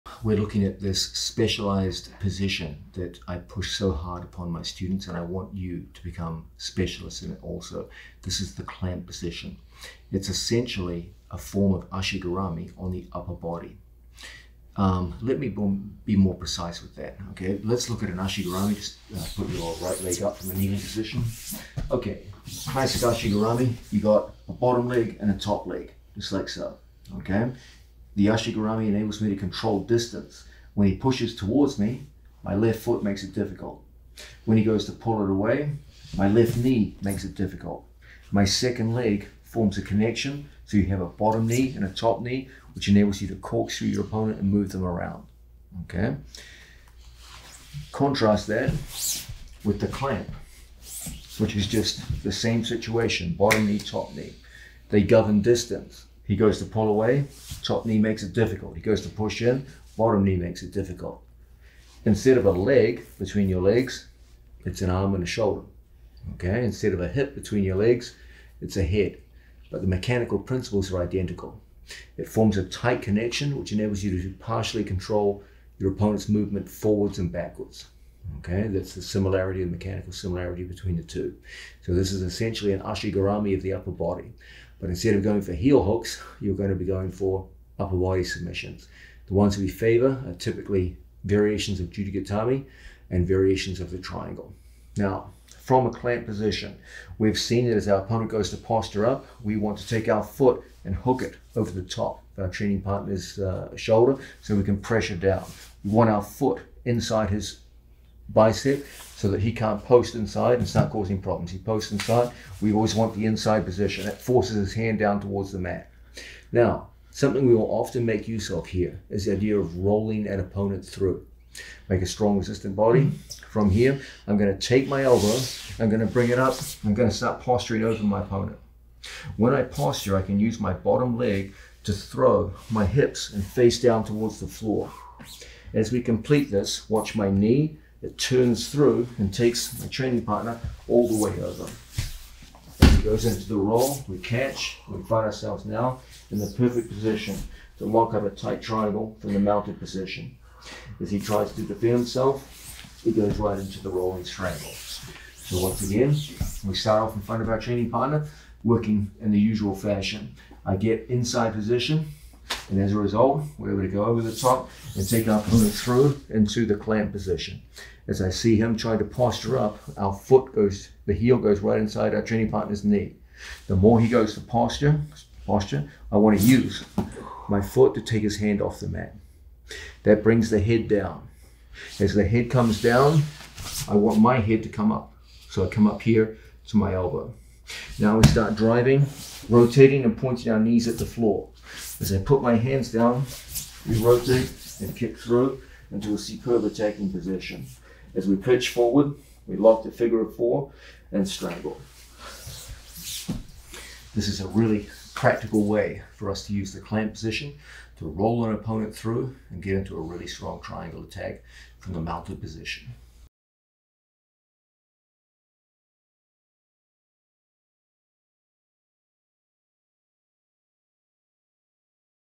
Rolling Triangle From the Clamp by John Danaher
ROLLING TRIANGLE FROM THE CLAMP

In this Jiu JItsu training video, John Danaher teaches the Rolling Triangle from the Clamp technique.

John Danaher is regarded as one of the most important figures in modern Jiu Jitsu history.  This is an excerpt from NEW WAVE JIU JITSU OPEN GUARD VOLUME TWO available from BJJFanatics.com.